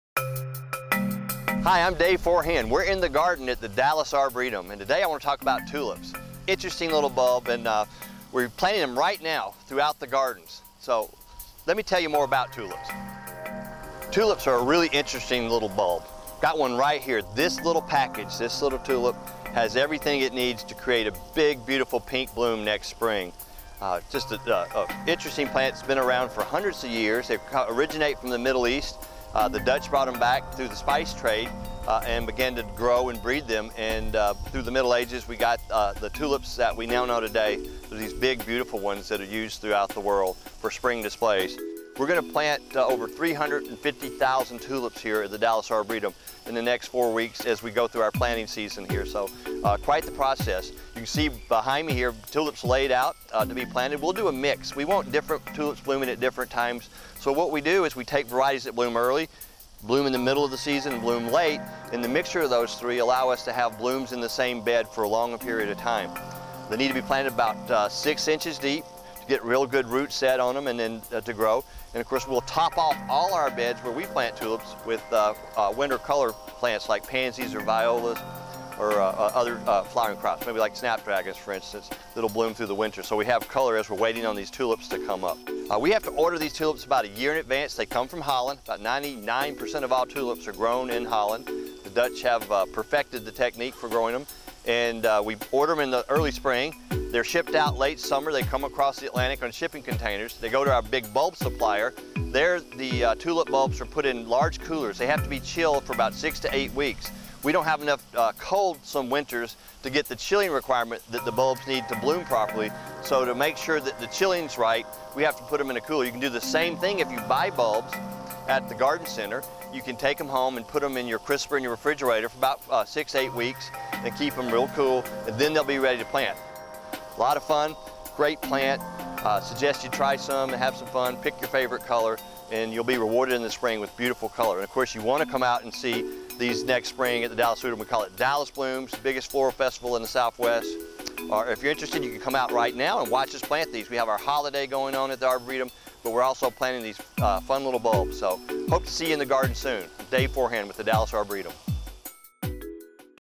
Blooms & Bulbs - In the Garden with Dave Forehand
Learn all about bulb planting at The Dallas Arboretum from Dave Forehand, VP of Gardens. We're planting 500,000 bulbs (350,000 just tulips) right now to get ready Dallas Blooms, our spring festival.